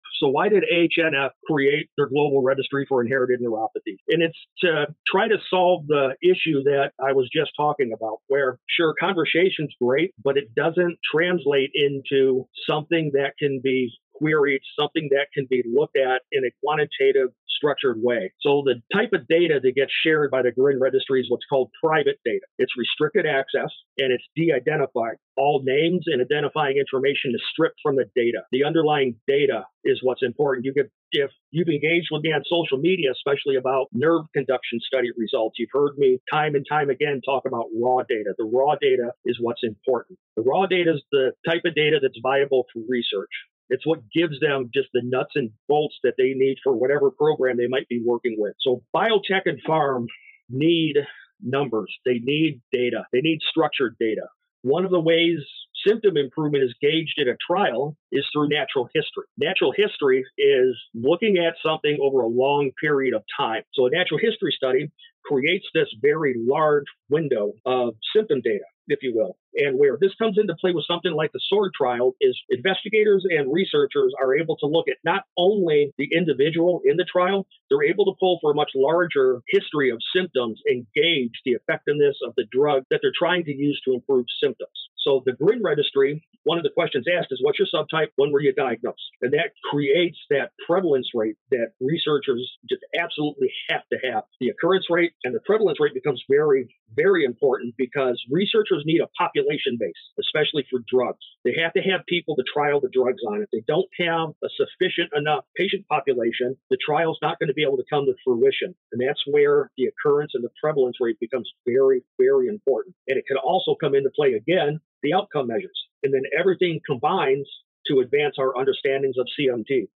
CMT Highlight Reel: How does CMT data drive research using GRIN?
Kenneth Raymond provides an overview on why sharing CMT symptoms on GRIN is so important!

The global registry for inherited neuropathy was created by HNF to address the issue of the lack of quantitative and structured data. The registry collects private, de-identified data that is valuable for research, particularly for biotech and pharma. The registry helps researchers to gauge the effectiveness of drugs by providing a large window of symptom data. The prevalence rates are important for researchers to have a sufficient patient population for trials. Ultimately, the registry helps to advance our understanding of CMT!